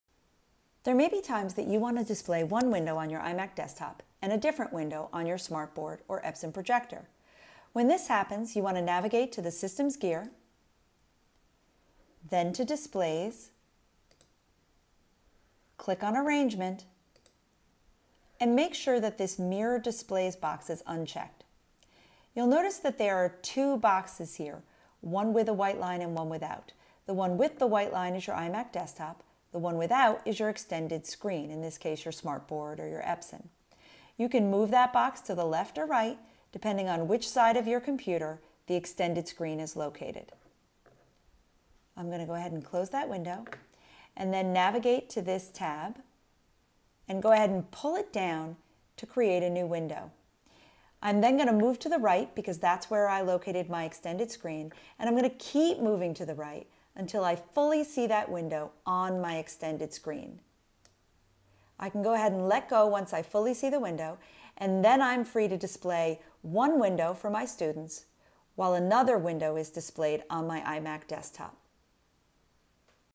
How to Extend Your Mac Desktop
There may be times when you want to display one window on your SmartBoard or a projected screen, and another on your desktop. This video will walk you through how to extend your screen to make that happen.